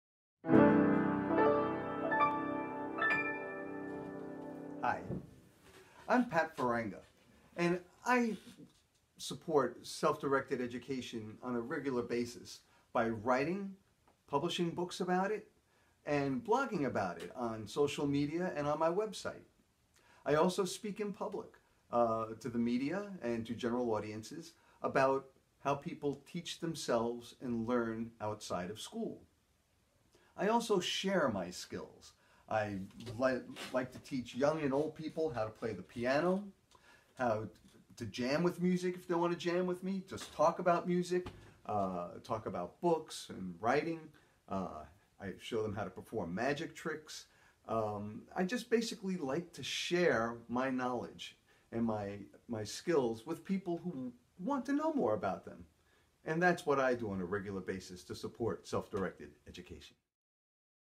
Pat Farenga - How Do We Facilitate Self Directed Education?
Part 2 of our 2018 selfiedirected campaign. We asked “How do you facilitate Self-Directed Education?”

Pat Farenga is a writer and education activist who brings more than 34 years of fieldwork, advocacy, and personal experience to the movement for Self-Directed Education. You can find some of his writing